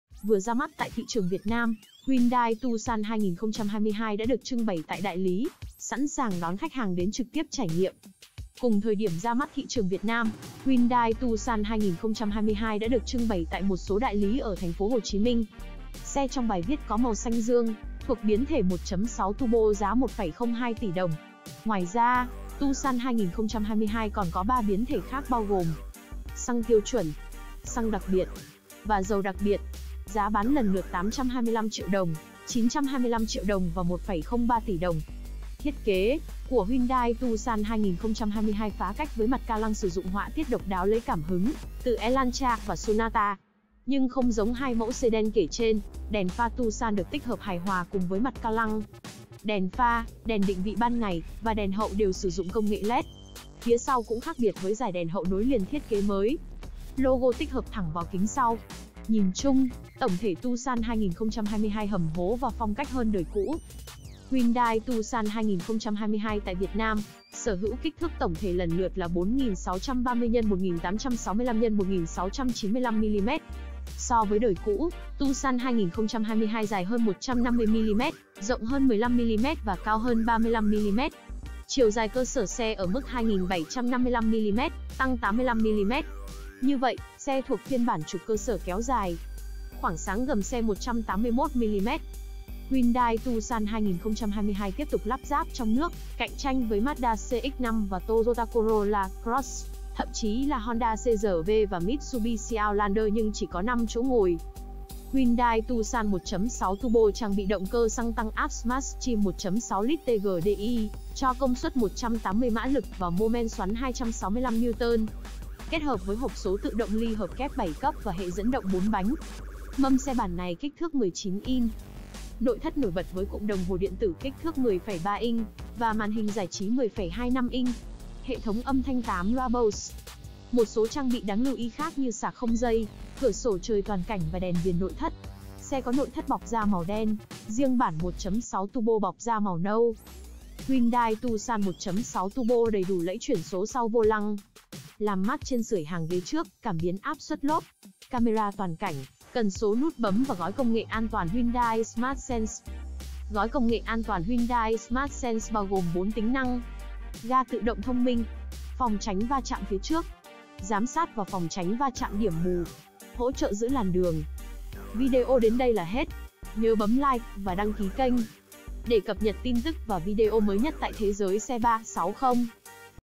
Hyundai Tucson 2022 tại đại lý : Đẹp mỹ mãn, nội thất khó chê. | THẾ GIỚI XE360
Hyundai Tucson 2022 tại đại lý : Đẹp mỹ mãn, nội thất khó chê.